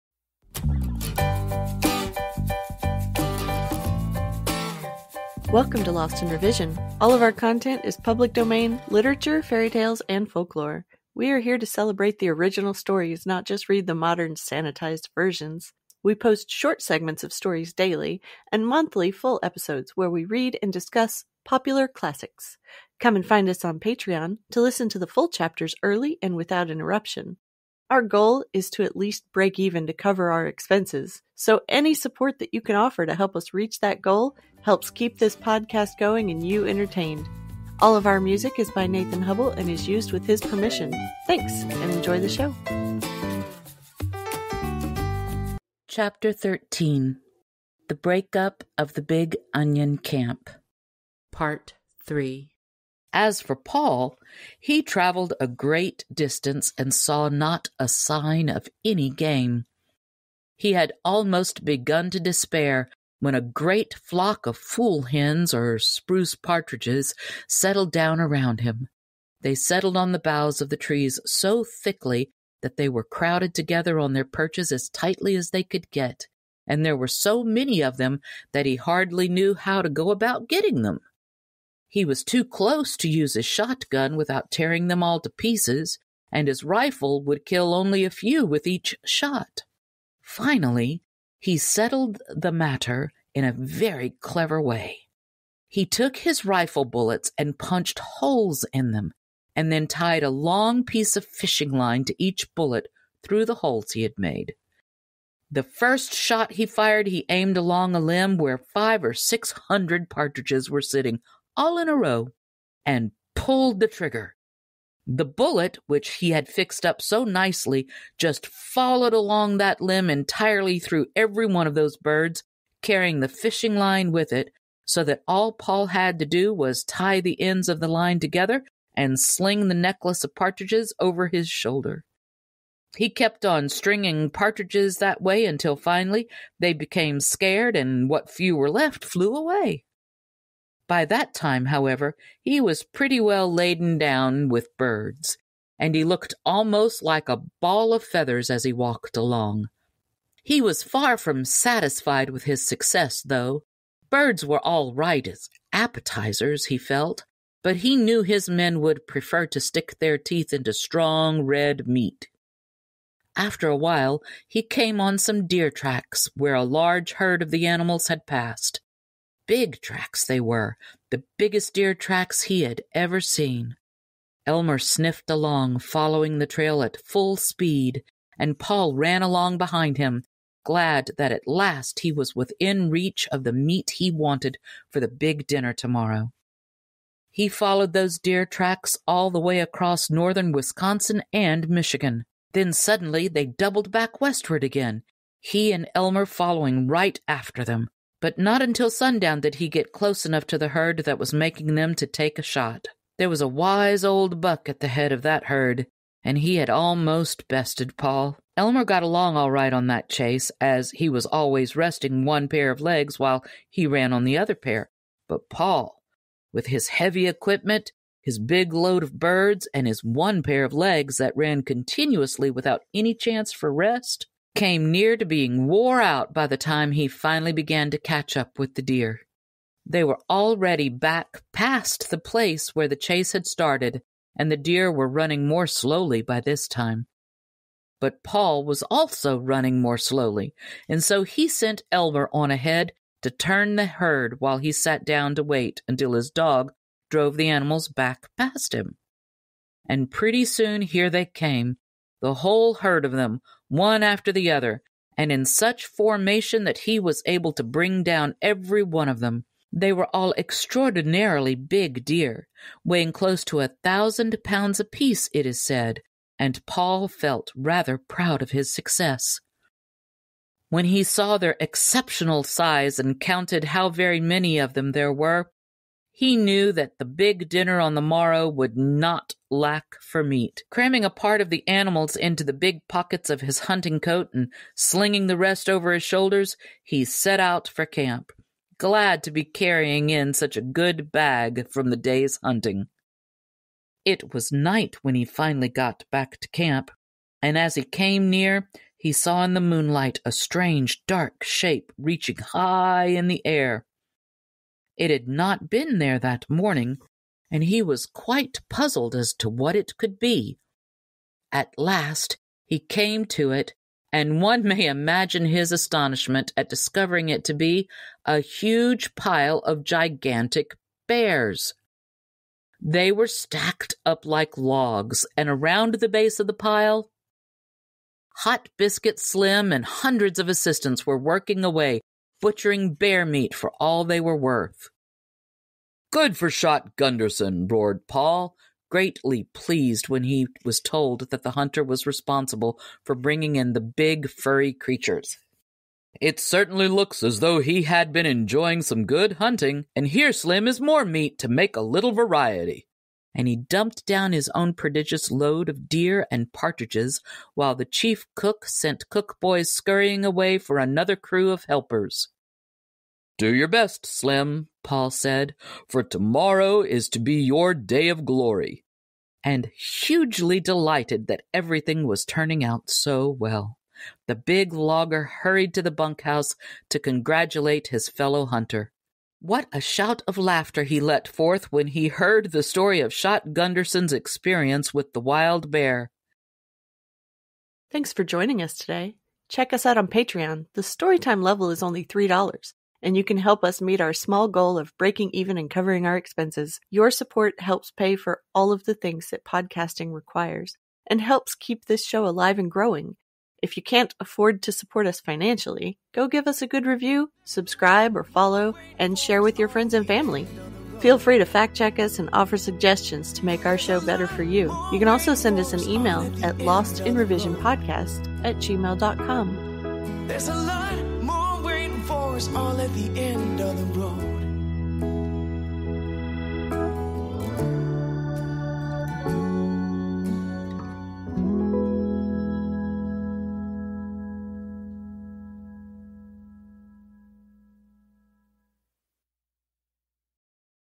Paul Bunyan and His Great Blue Ox Ep 44 | Lost in Revision
Paul Bunyan and His Great Blue Ox Episode 44

By: Wallace Wadsworth

Read by: Angelina Adams

Chapter 13: The Break-Up of the Big Onion Camp